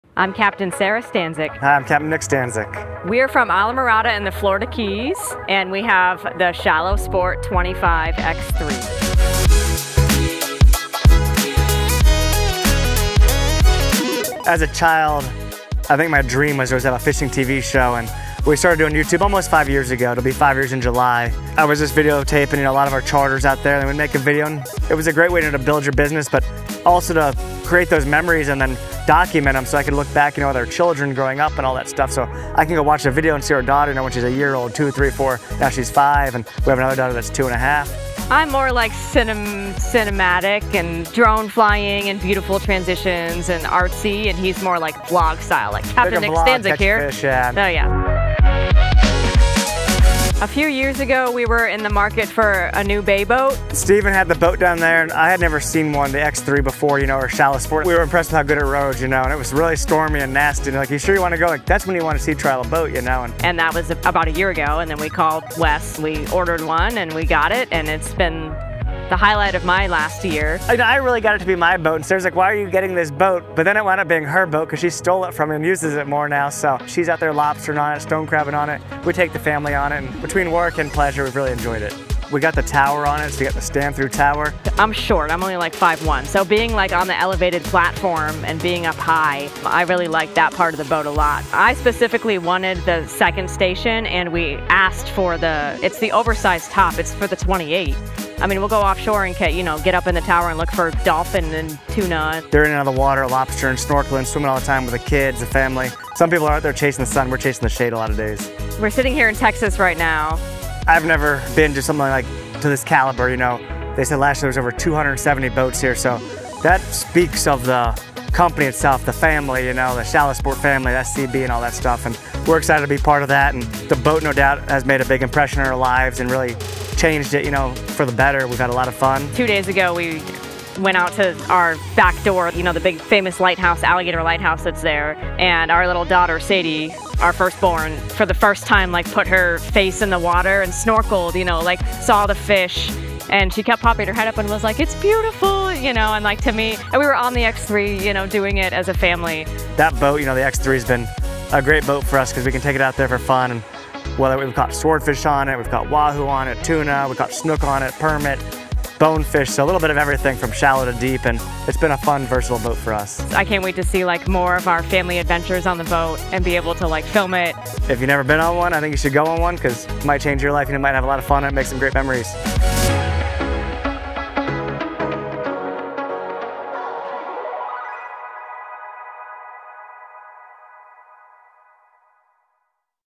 StanzFishing - 25' X3 by Shallow Sport Boats
Pro Teamers, Capt. Nick & Sara Stanczyk talk about their 25' X3 while at our 2023 Shallow Sport & SCB Owners Tournament on South Padre Island, TX.